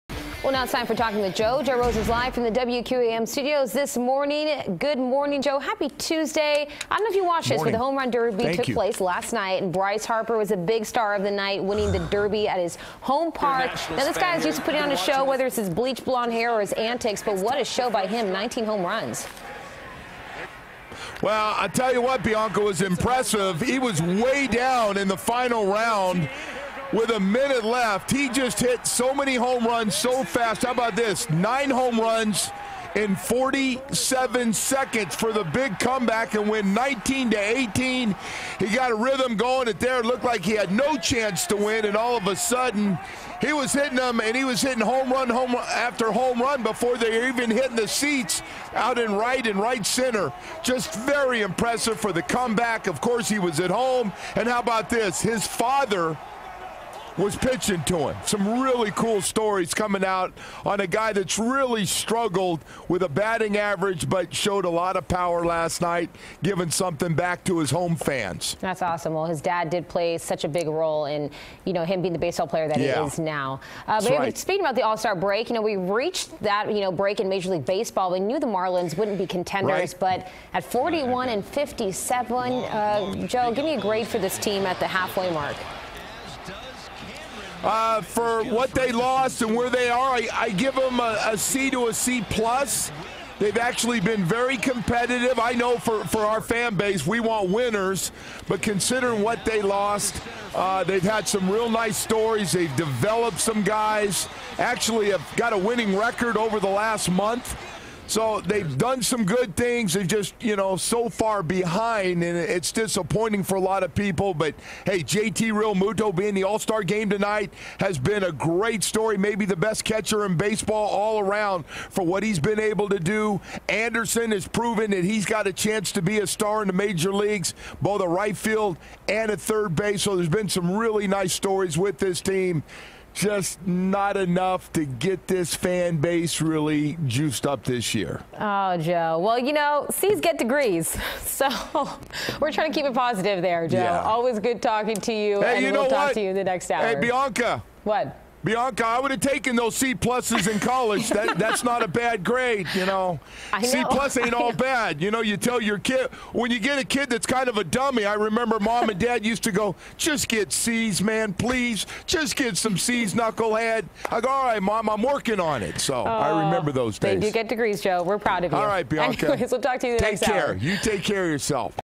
Talkin' With Joe 7/17
WQAM's Joe Rose grades the Marlins now that we've reached the half-way point with the All-Star break.